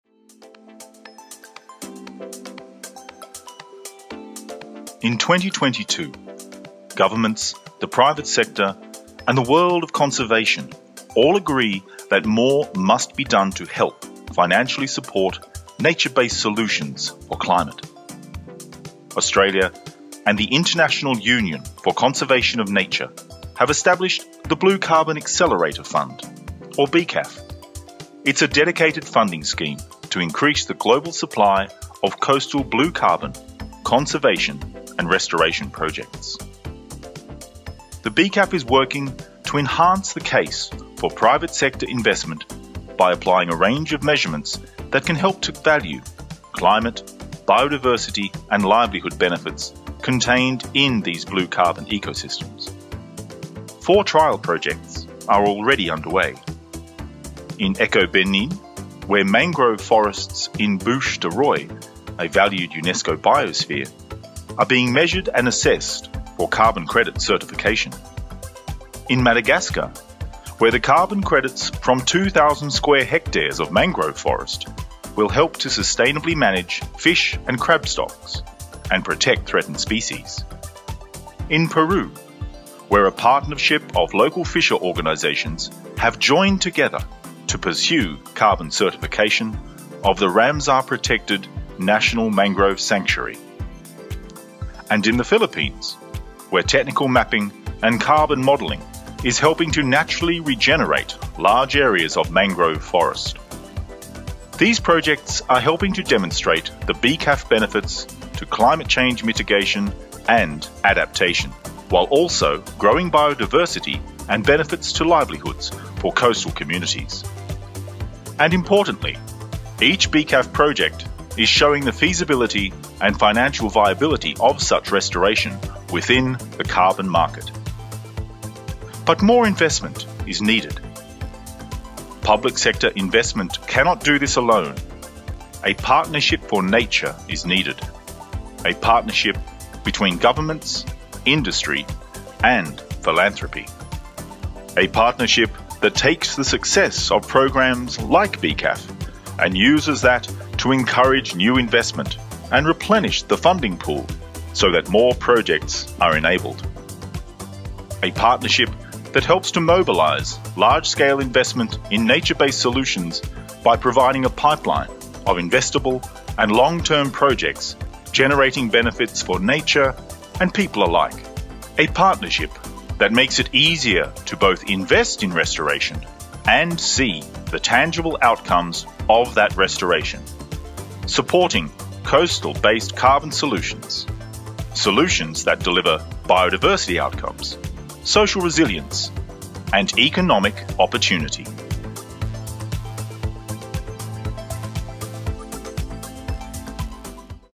The Blue Carbon Accelerator Fund
The Blue Carbon Accelerator Fund (BCAF) supports the development of blue carbon restoration and conservation projects in countries outside Australia and help pave the way for private sector finance. Find out more about blue carbon at the International Partnership for Blue Carbon